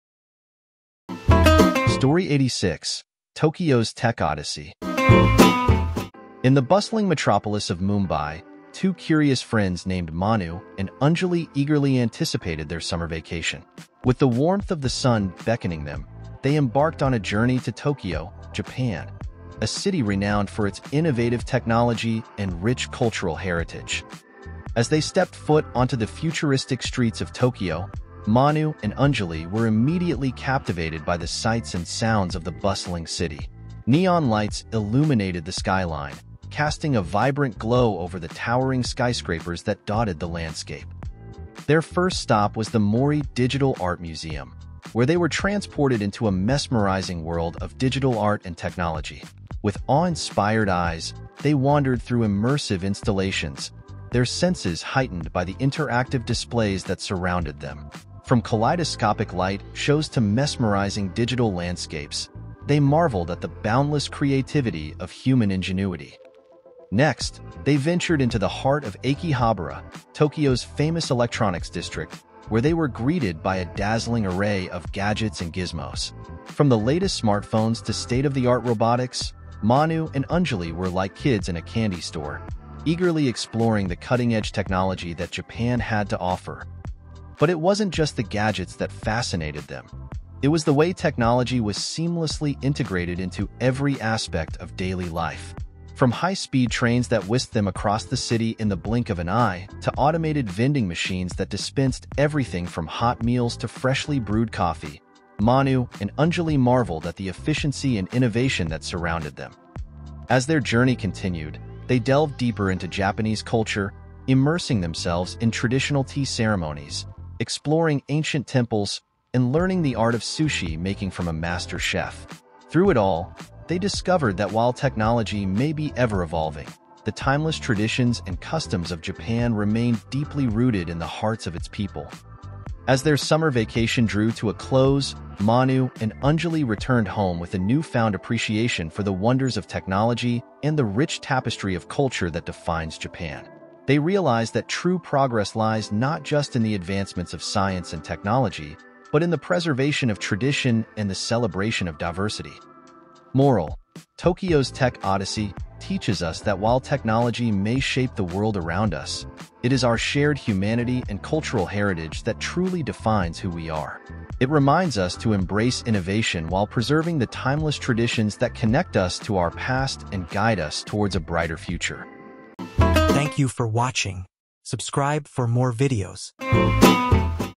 “Tokyo's Tech Odyssey”| English Story | Bedtimes | Moral Stories | Ai Animated | ES-86
Join Manu and Anjali on an awe-inspiring journey through Tokyo, Japan, as they explore the city's cutting-edge technology and rich cultural heritage. From immersive digital art museums to the bustling streets of Akihabara, experience the wonders of Japan's tech landscape and the timeless traditions that define its people. Embark on this unforgettable adventure and discover the harmony between innovation and tradition in "Tokyo's Tech Odyssey.“